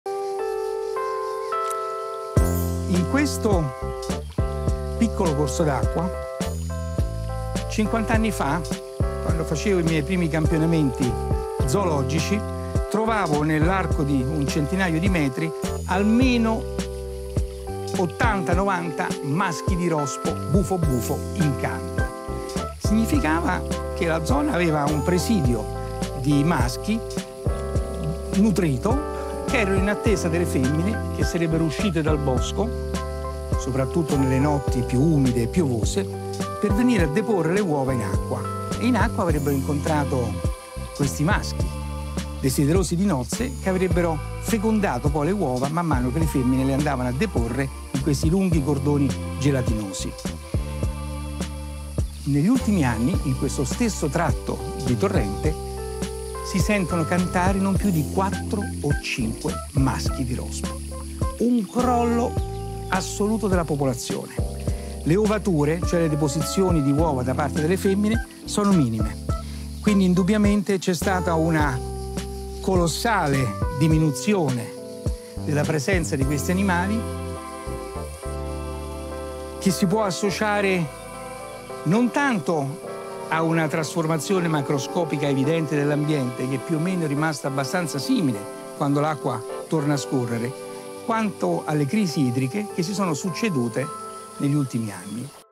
Il declino dei rospi
Si sta verificando una diminuzione della presenza di questi animali che si può associare alle crisi idriche che si sono succedute negli ultimi anni.